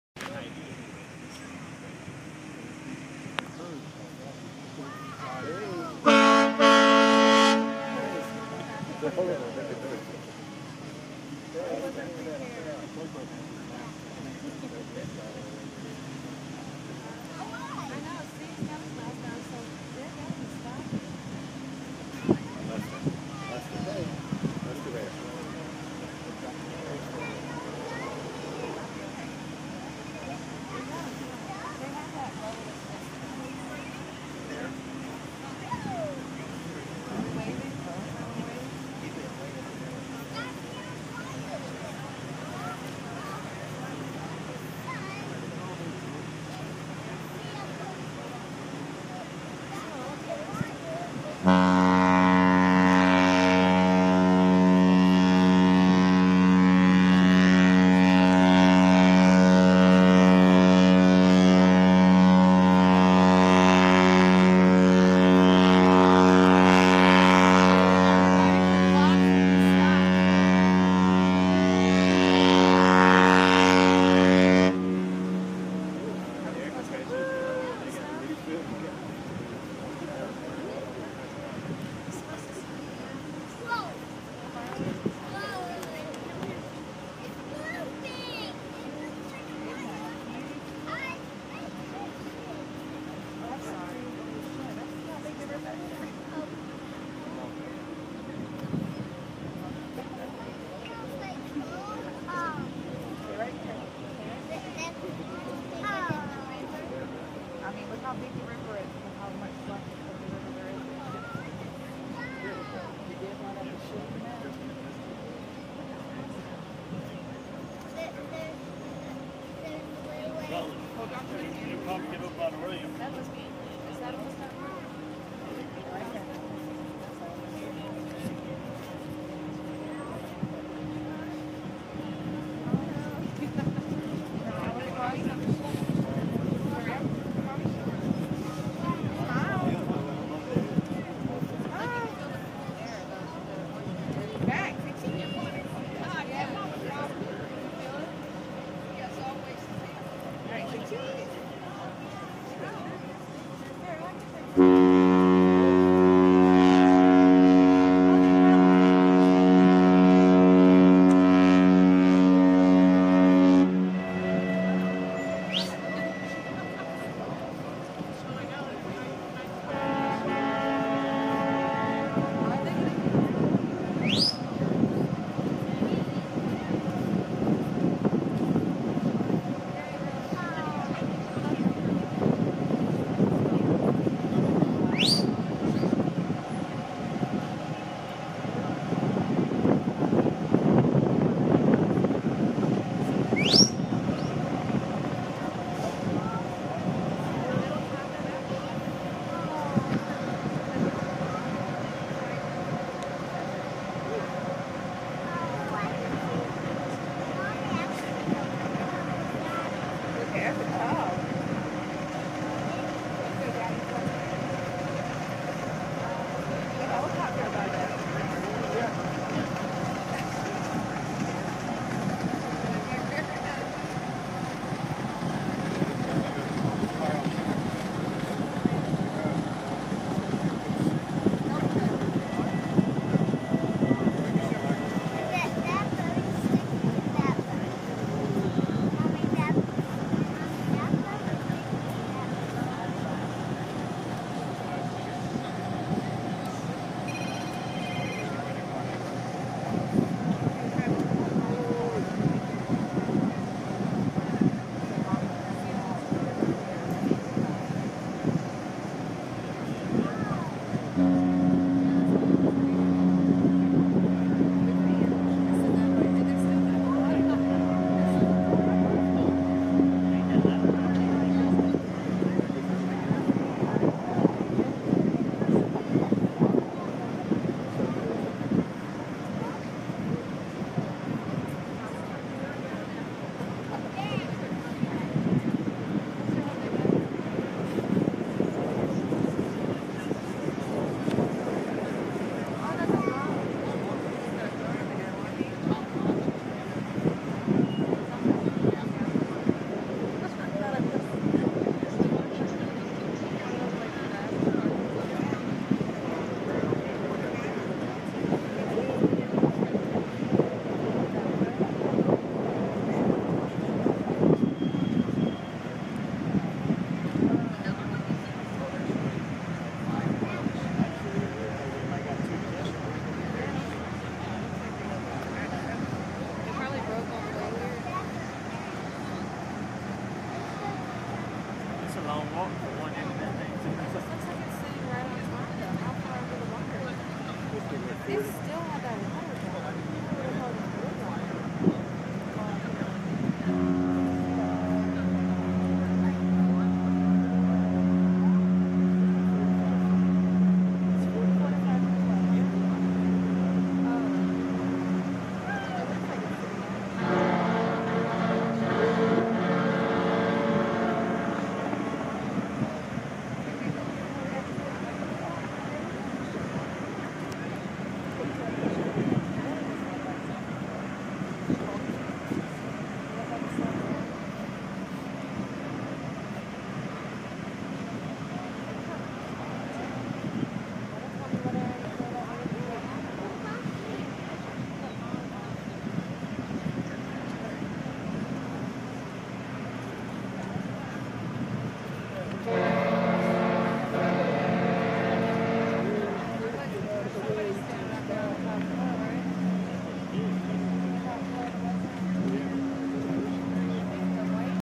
Largest container ship to ever call on the US East Coast arrives in Savannah, Ga.
Largest container ship ever to arrive on the East Coast sailed onto the Savannah River on May 11, 2017. The Cosco Development is the length of four football fields and nearly 160 feet wide.